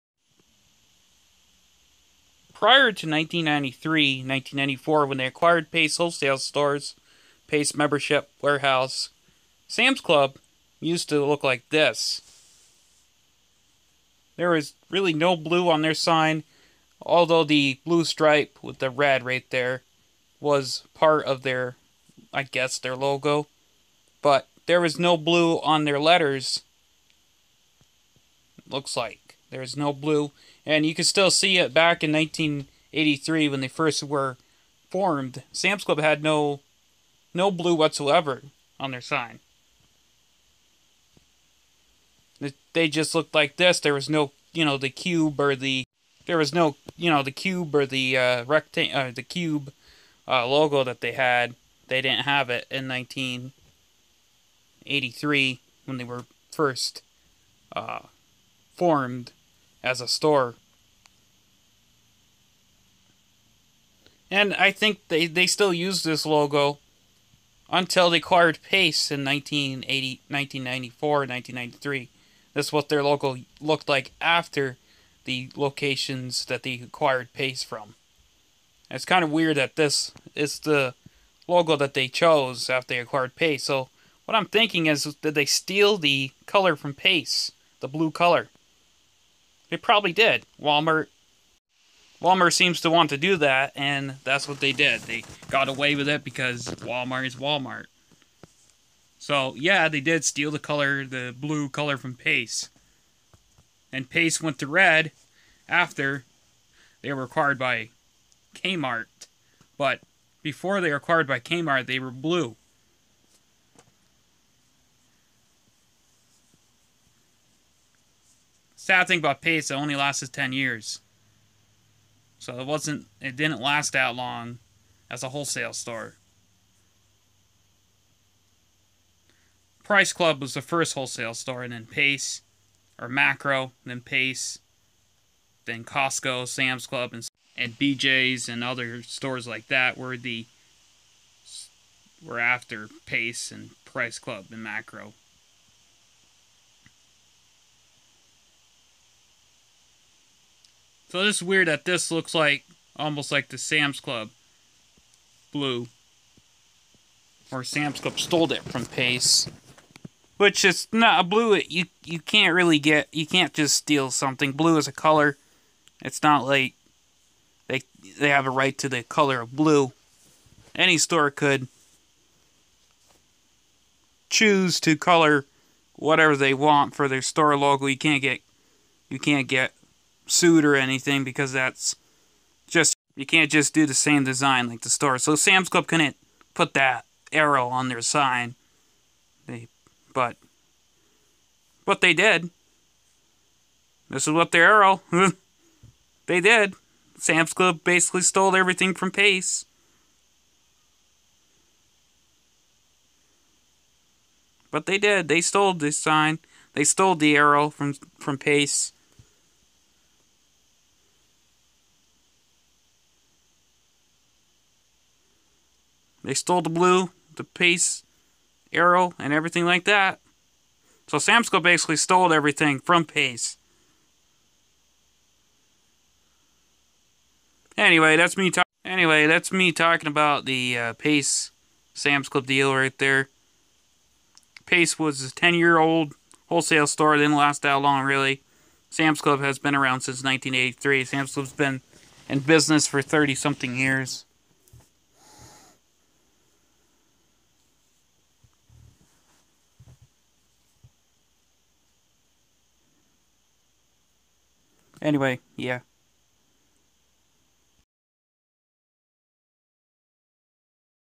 Sams club took Pace stores logo design etc well thats Walmart for ya
pace membership warehouse from 1983 to 1994 was a somewhat Sam's club type store it operated in many states until the 1994 buyout from Walmart to be turned into Sam's club stores Sam's club took their logo and blue paint